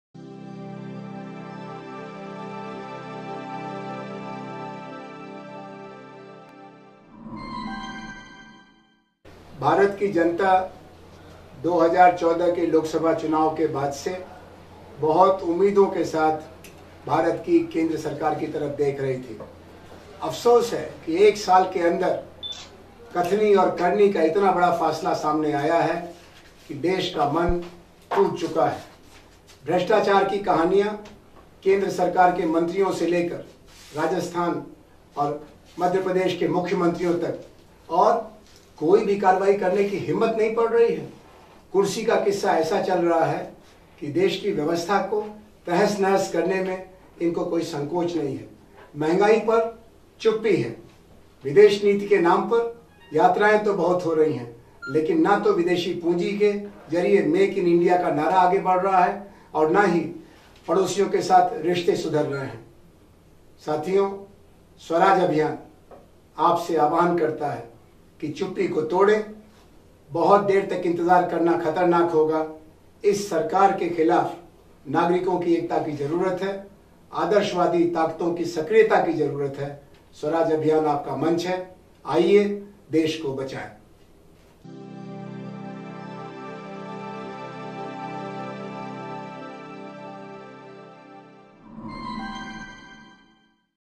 Swaraj Abhiyan is your platform - Join to save nation - Nation Needs you most - Anand Kumar.
The people of the country voted a Govt that came to power on high promises but it is over an year now. Nothing has changed on the contrary things of deteriorated.  In just over one year the stories of corruption have surfaced with such shame from its Ministers to its governments in MP and Rajastan. And the worst part is the Govt has no courage or sincerity to take action on any of them. The love of power and authority is such dear no one likes to take any action on the guilty.On the other hand on prize rise a deaf silence, in the name of foreign policy many foreign tours are made but in reality nothing is seen in reality in terms of advancement in our foreign investments pour in nor the slogan of make in India is showing signs of development. And nor our relations with the neighbors are improving. Friends please break your silence come out in protest of this govt. India today needs like minded and idealistic people to join and unity against all wrong by this govt. And it is time to unity all right people to save the nation.  Join Swaraj Abhiyan.